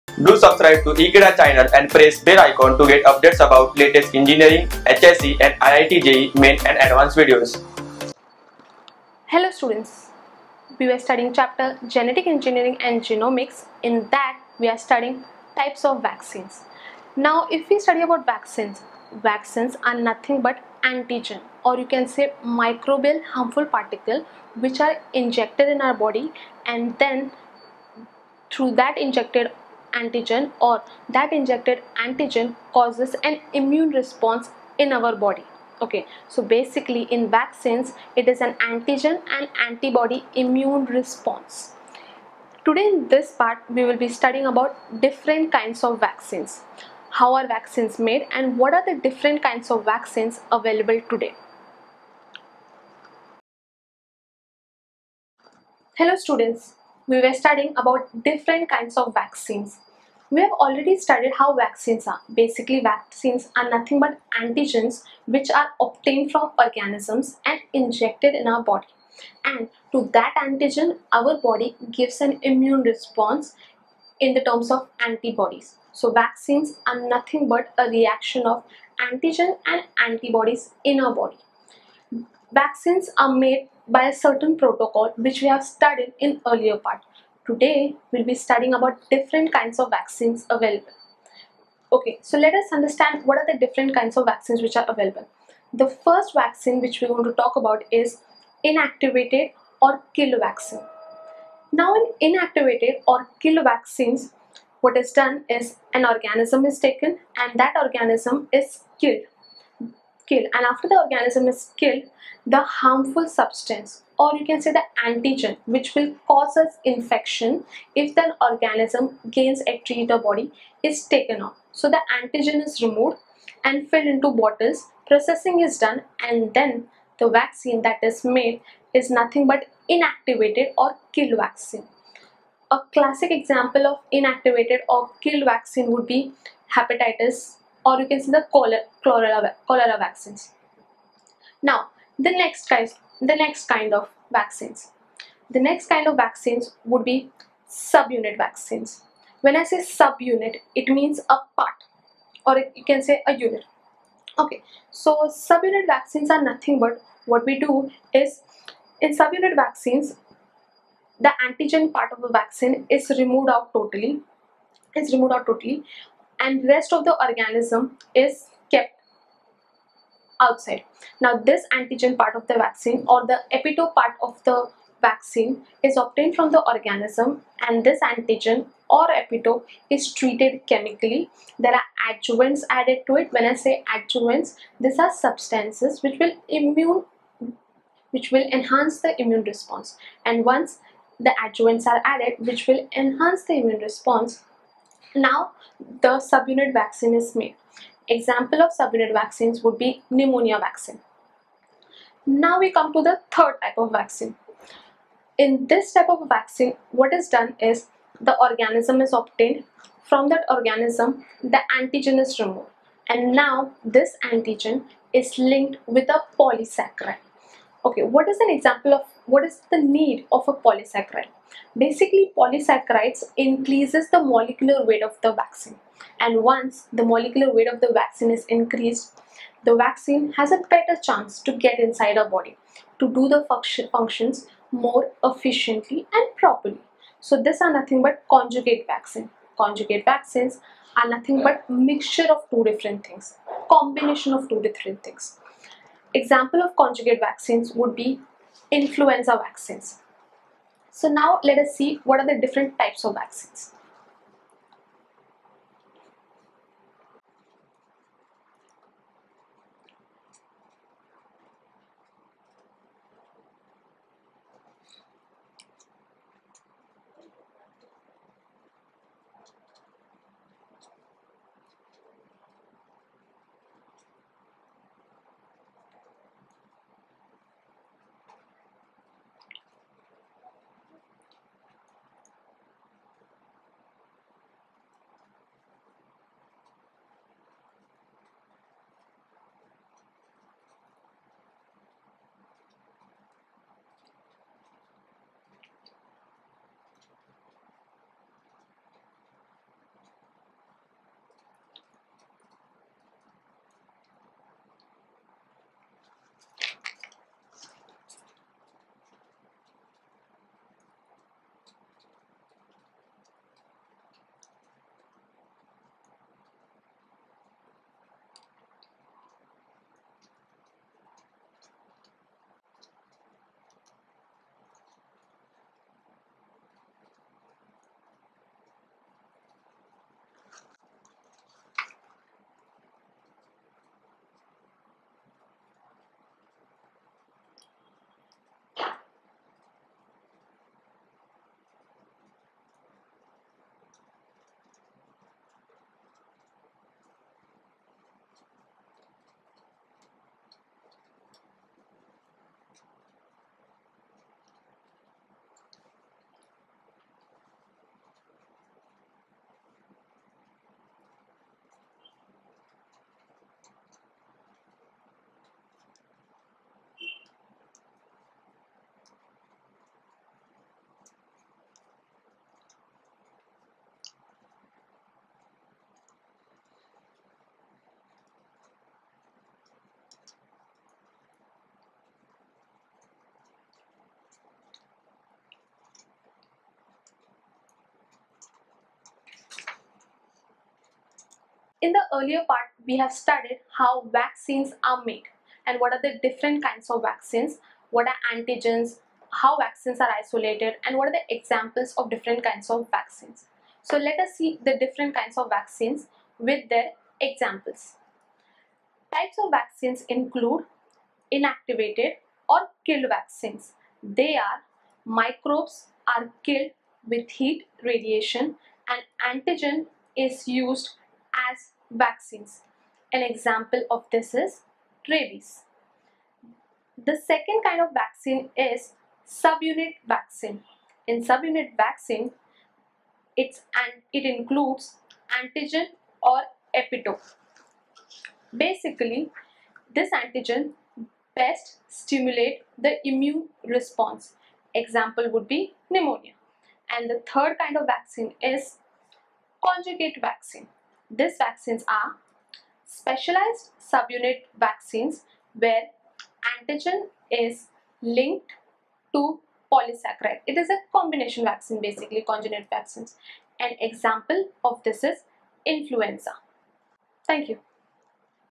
Types of Vaccines - Genetic Engineering and Genomics - Biology Class 12
Video Lecture on Types of Vaccines from Genetic Engineering and Genomics chapter of Biology Class 12 for HSC, CBSE & NEET.

Watch Previous Videos of Chapter Genetic Engineering and Genomics:-

Happy Learning : )